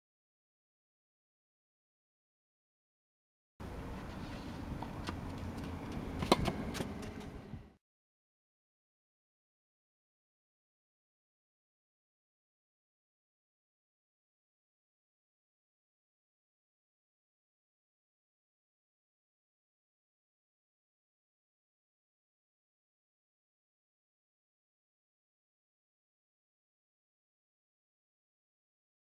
あなたも打てるスーパーショット ～股抜き～ スマッシュ2014年12月号
テニス雑誌月刊スマッシュ2014年12月号では、中とじ特別企画として『あなたも打てるスーパーショット』（解説：白田浩史コーチ）を掲載しています。本誌で打ち方を詳しく解説しているショットの動画をこちらにアップ。スローも入っていますので参考にしてください。